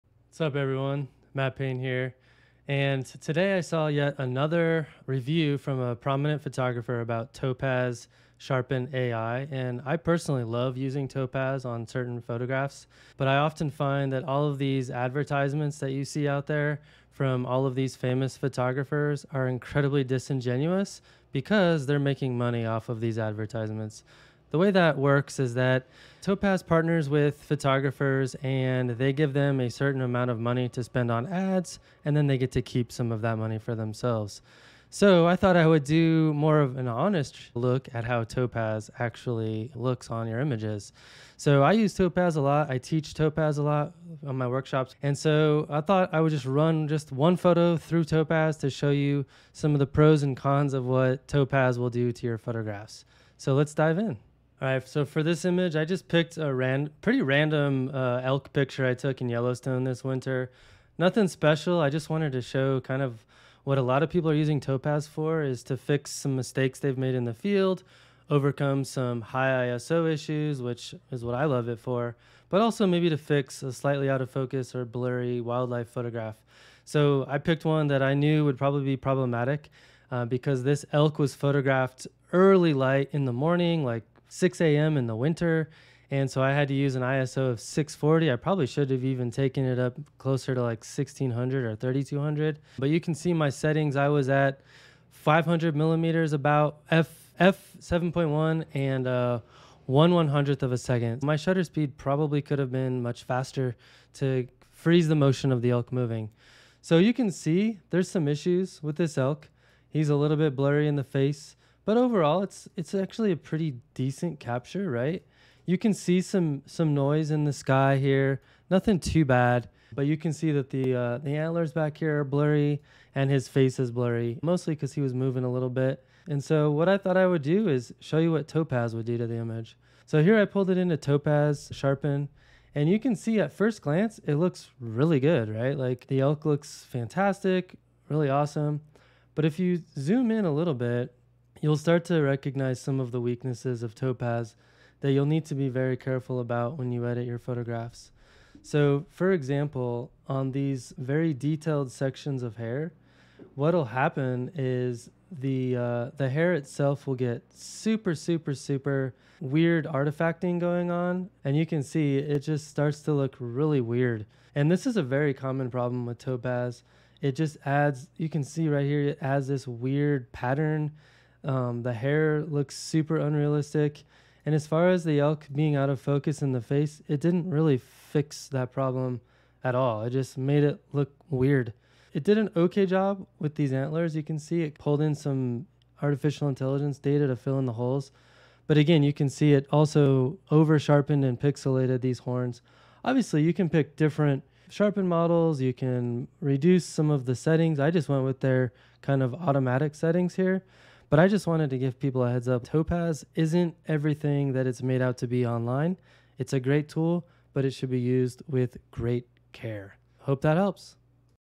An Honest Look at Topaz AI
I love using Topaz AI for my photography editing, but I'm a bit tired of all of the disingenuous ads you see on social media heralding the software as some kind of magic elixir that will instantly fix your photographs. So, I decided to make quick video to showcase some of the strengths and weaknesses of the software. I could have probably done a 2 hour video to convey my point and to showcase other methods of fixing these problems.

As an aside, if you do use Topaz, I recommend doing it through Photoshop, duplicating your original layer, and then running Topaz on the duplicated layer. You can then use your original layer to mask back in any mistakes or oddities that Topaz introduces through its algorithms.

PS if you want to check out Topaz, you can find it

I get NO money from Topaz, so you can trust my review to be a bit more honest.

That's another problem I see with YouTube in general - a lack of honest reviews lacking in corporate influence... I digress.